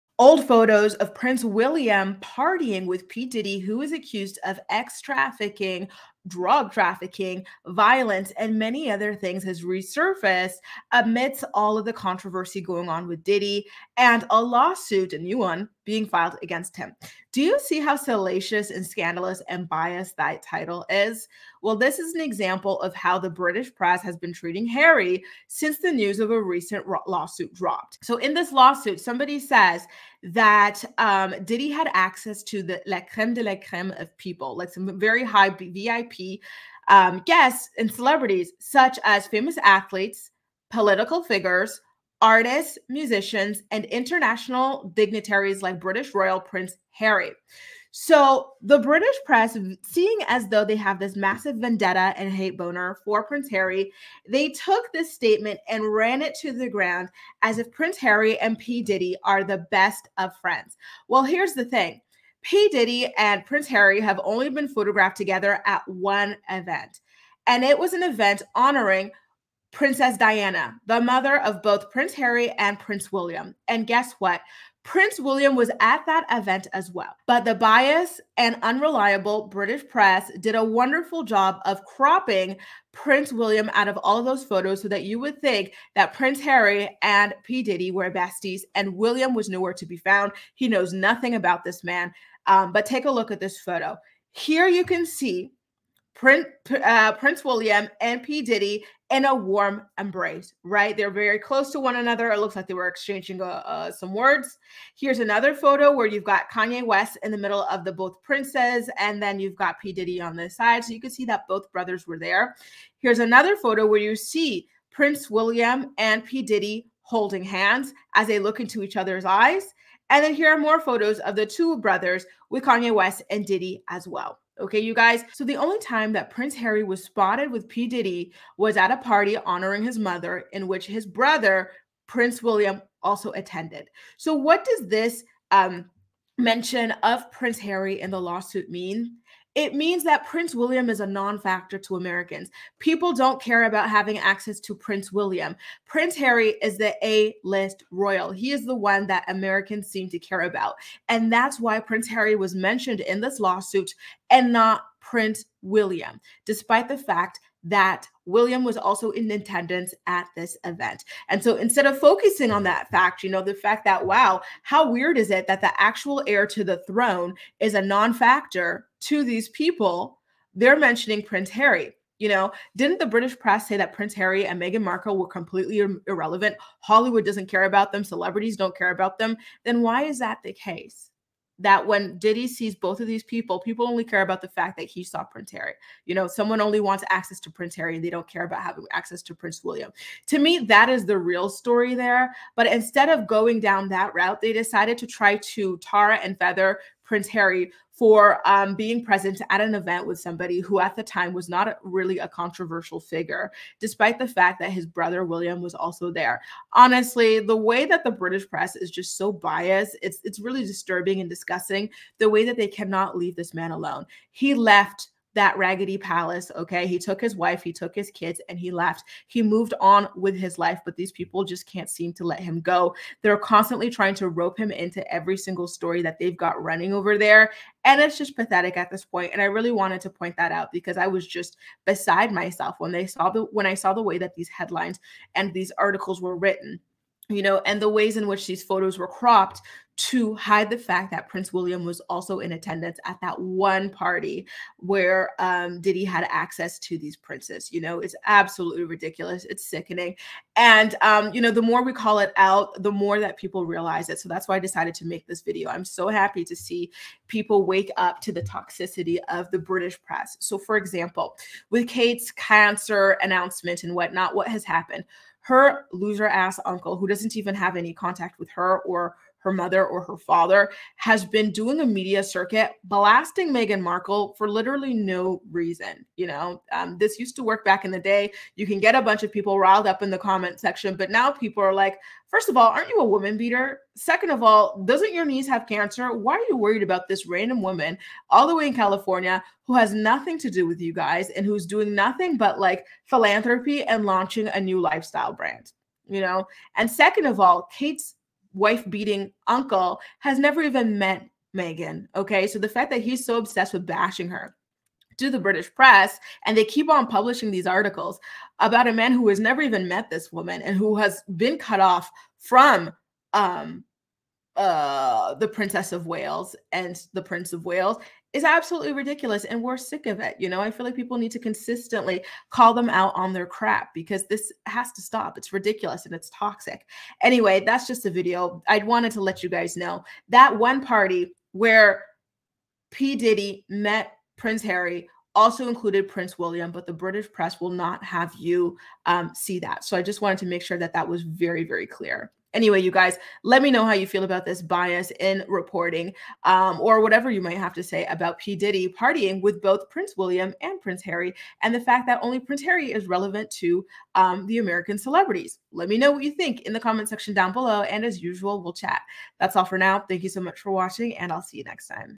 Prince William & Diddy's EPIC PARTY PHOTOS RESURFACE Amidst Lawsuit Drama!
Let's talk about the biased media reporting in regards to Prince William & Prince Harry's associations with disgraced rapper P. Diddy.